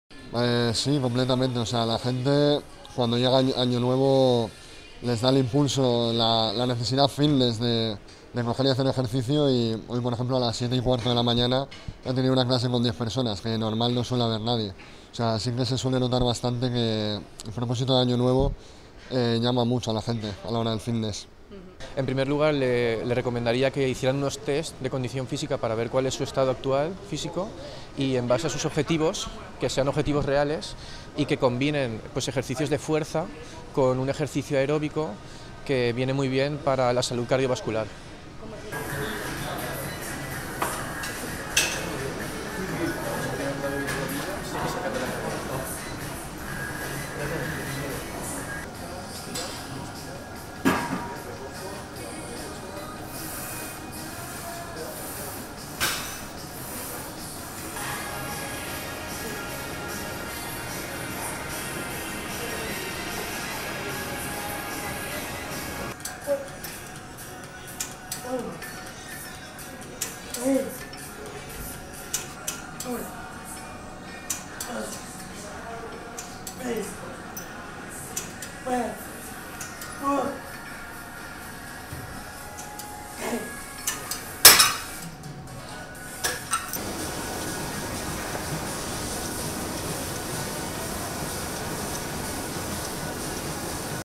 Gimnasios notan un mayor "ímpetu" deportivo a principios de año
El gimnasio Atalanta Sports Club & Spa de València ha registrado un mayor "ímpetu" entre usuarios y nuevos clientes por hacer deporte a principios de año para "quitarse el peso que han ganado en las cenas y comidas familiares de Navidad". El equipo de entrenadores personales han aconsejado plantearse "metas más realistas" en los propósitos de año nuevo y así mantener la motivación deportiva "más a largo plazo".